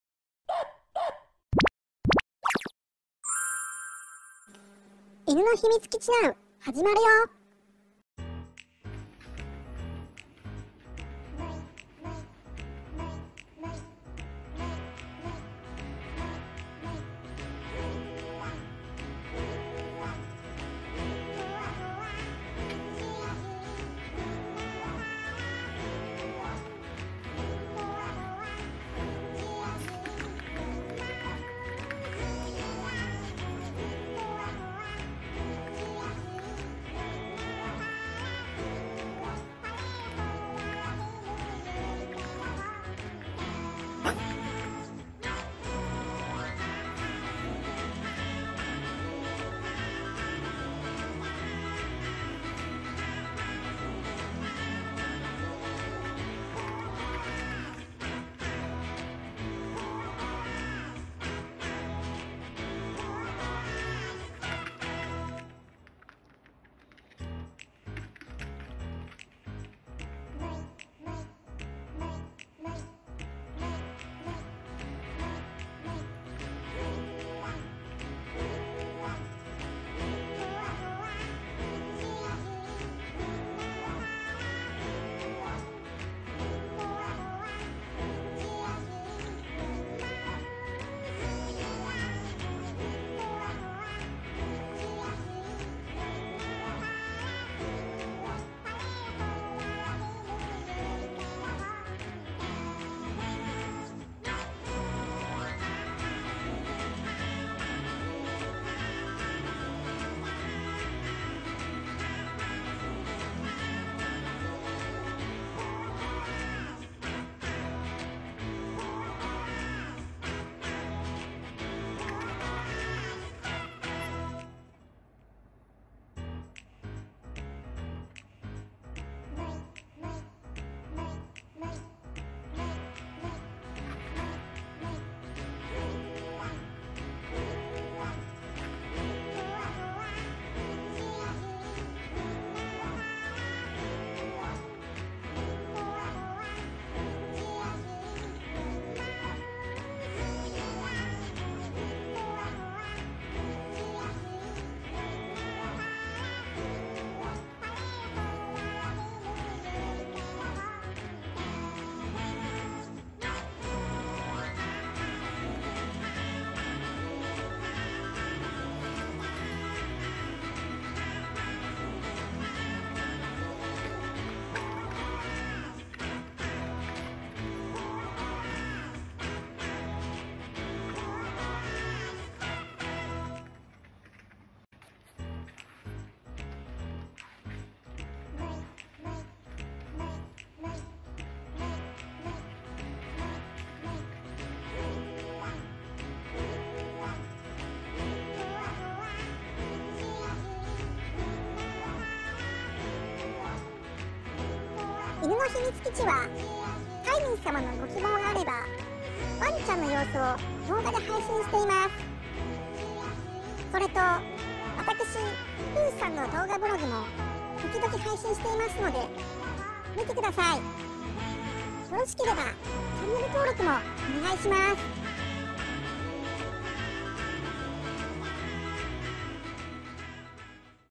のんびり月曜日 🐶ひみつきちなう！🐶 (チワワ 柴犬 ビションフリーゼ トイプードル) 2020年3月2日
新型コロナもありますし…お部屋でゆっくりしましょう。

ビションフリーゼ　もちこちゃん
トイプードル　るなちゃん